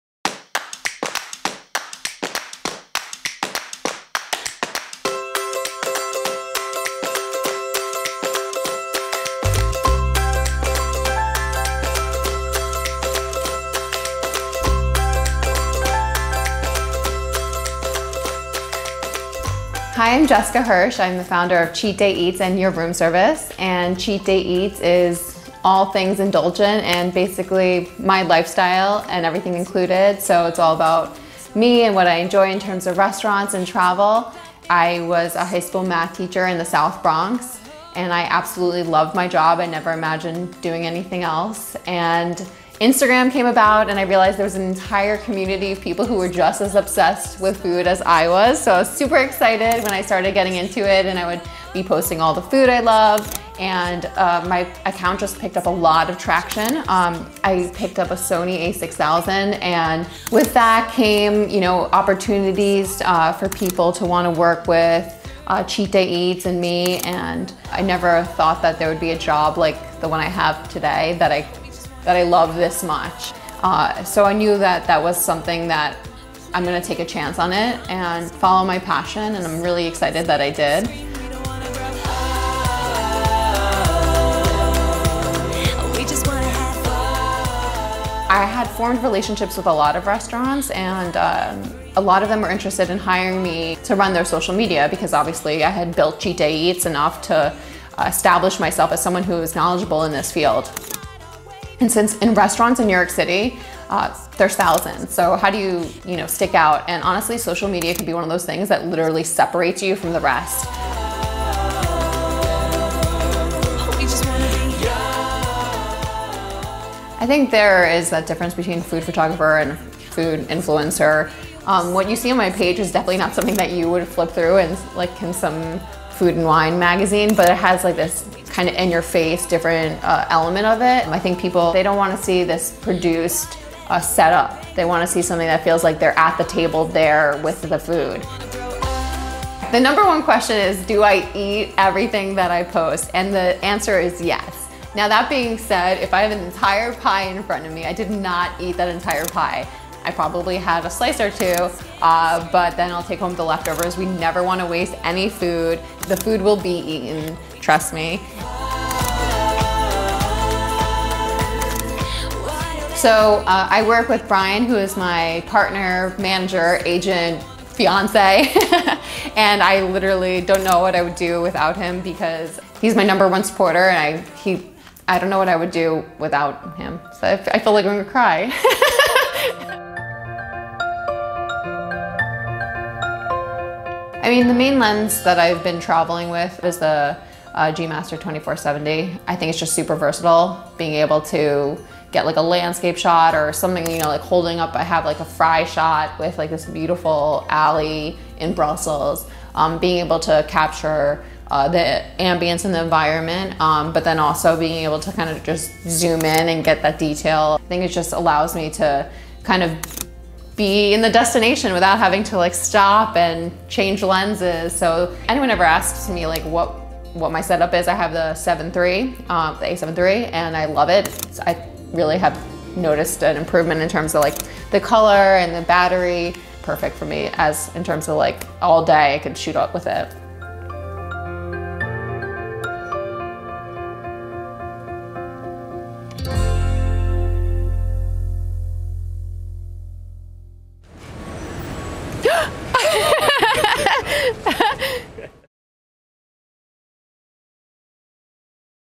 Sony Alpha | Food Photographer Jessica Hirsch (@cheatdayeats) - We Are Alpha
As the founder of @cheatdayeats & @yourroomservice, Jessica Hirsch's in-your-face food photography style has gained a major following on Instagram. See how this Alpha Collective member brings her food shots to life and uses social media to separate them from the rest.

@yourroomservice (luxury travel)

Shot and Edited by Sal D'Alia